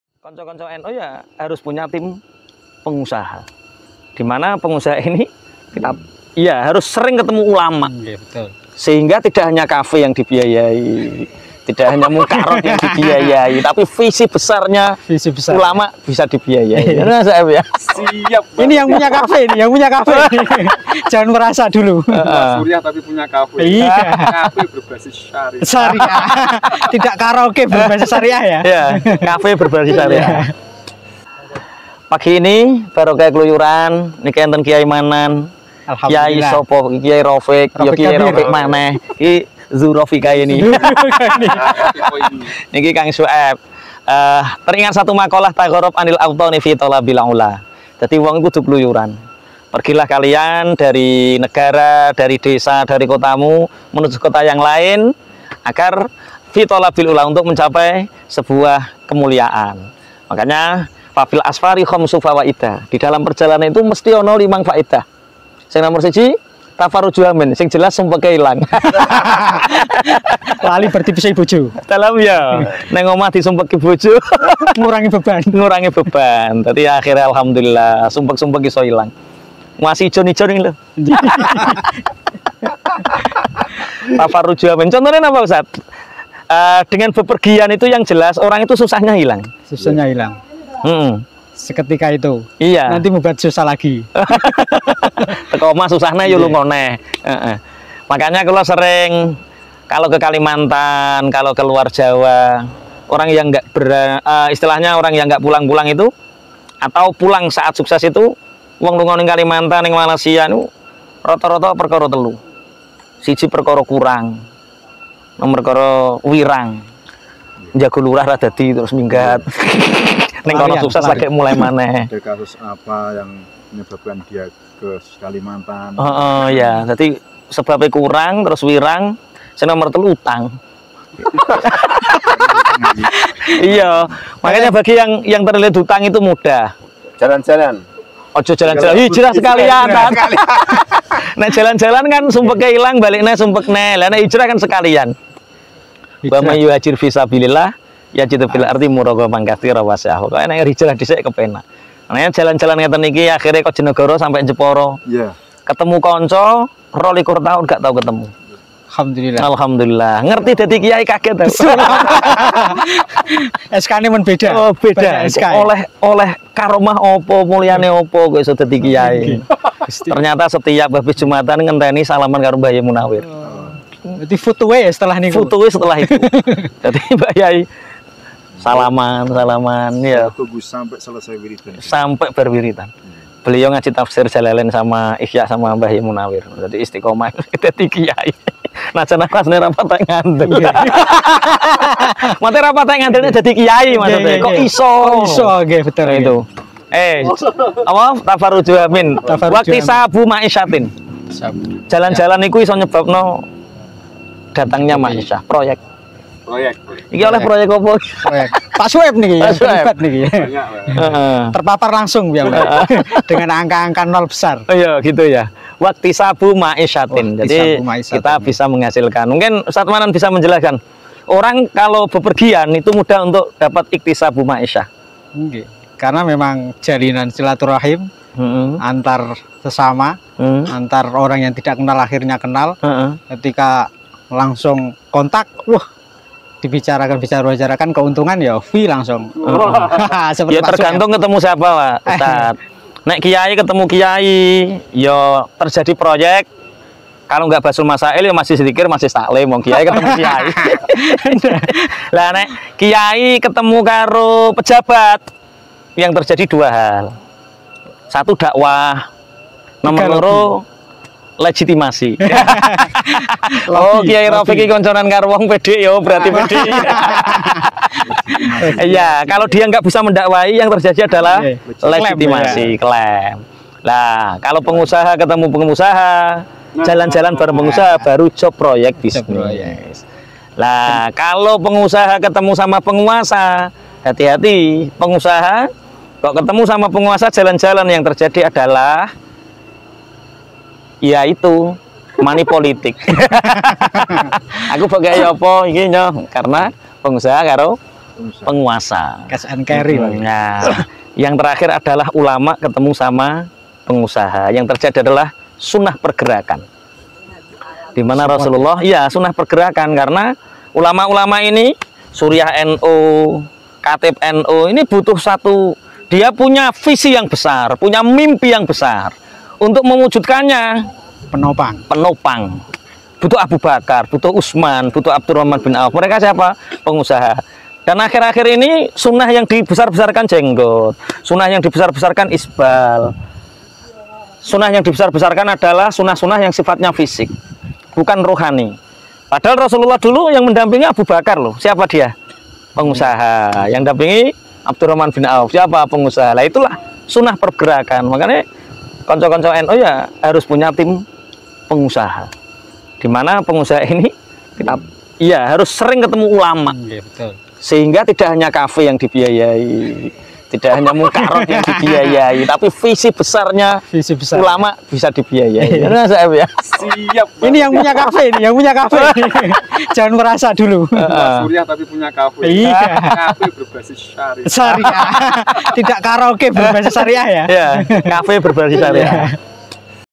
KH.Toha Abrori ( Santri Senior ) Di Somosari Jepara - Hikmah Kluyuran
KH. Toha Abrori atau yang biasa di kenal dengan Santri Senior adalah pengasuh dari ponpes khozinatul abror bojonegoro jawa timur.

beliau sempat hadir ke jepara dalam rangka berkunjung ke tempat teman beliau sewaktu mondok dulu di tangir jawa timur bapak syoeb yang kebetulan juga ketua LTN MWC NU Batealit

Dalam pertemuannya beliau membuat konten jagongan ala santri tepatnya di puncak bukit Somosari di bawah lereng gunung muria dengan pemandangan yang begitu indah memukau .